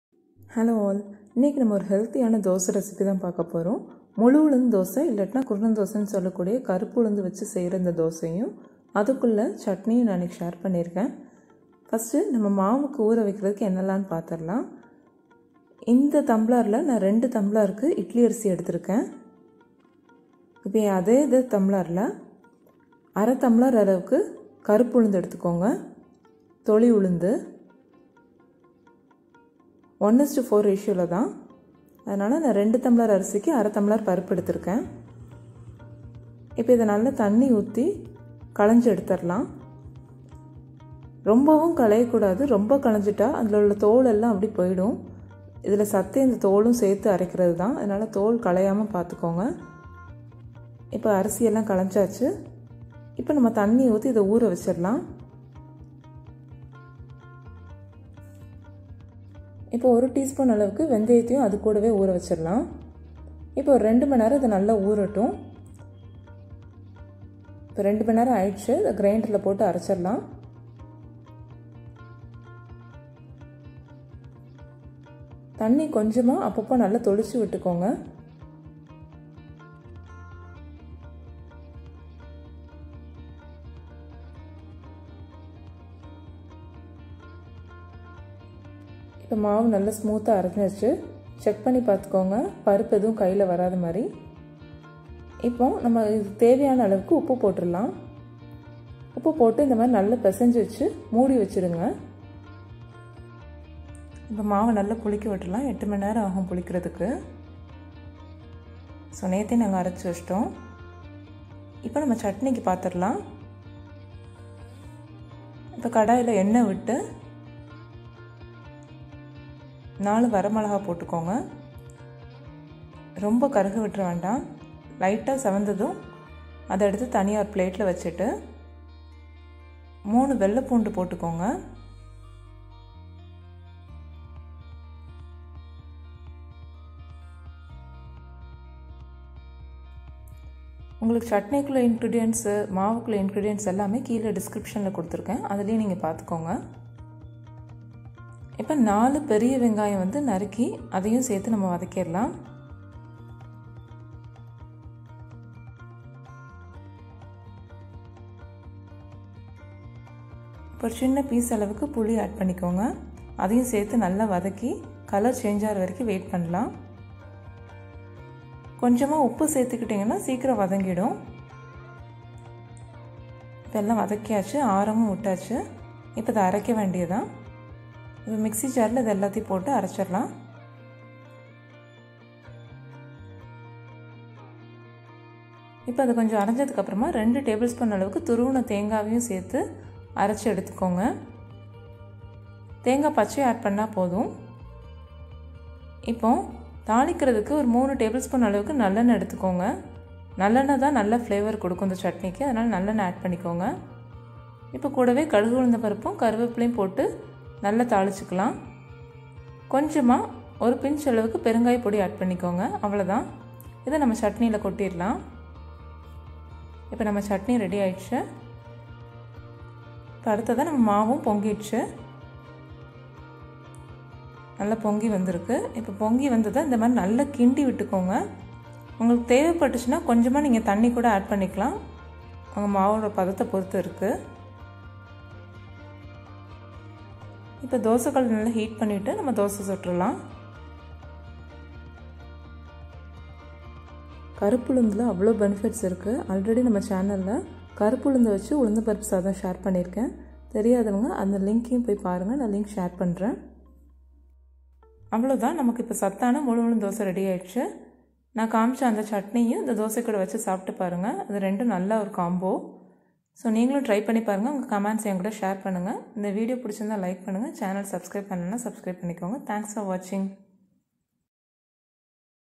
கருப்பு உளுந்து தோசை || முழு உளுந்து தோசை || கருப்பு உளுந்து recipes
This video is about black urad dal dosai which is known as muzhu ulundhu dosai or kurunai dosai. Generally black urad dal have many health benefits.include this recipe to your diet and stay healthy. thank you :)
INGREDIENTS:
FOR BATTER:
idly rice : 2 glass
black urad dal - 1/2 glass (1/4 + 1/4)
fenugreek seeds - 1tsp
salt
FOR CHUTNEY:
garlic - 3
a piece of tamarind
salt
SEASONING: (GINGLEY OIL ,MUSTARD SEEDS,CURRY LEAVES,HING)